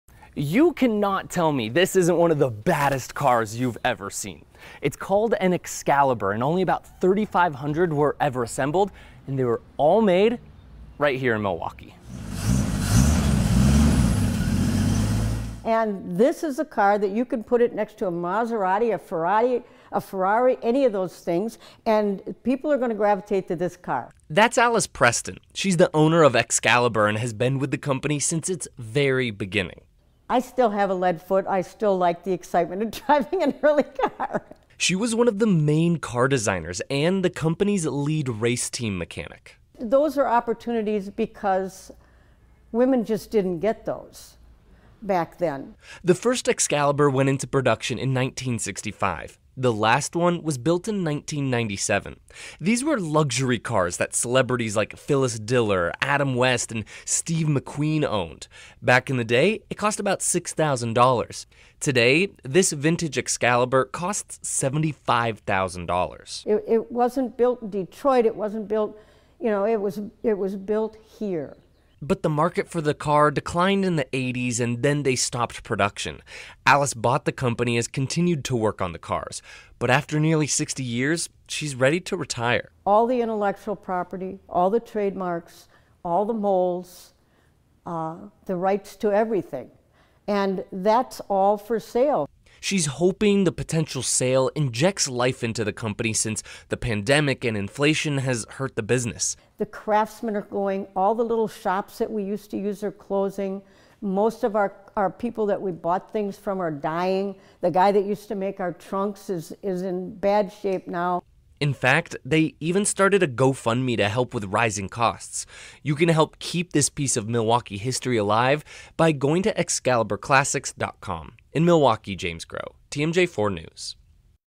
Excalibur, the iconic luxury car that was made in Milwaukee
One of the coolest luxury cars ever made that celebrities loved to buy was made in Milwaukee.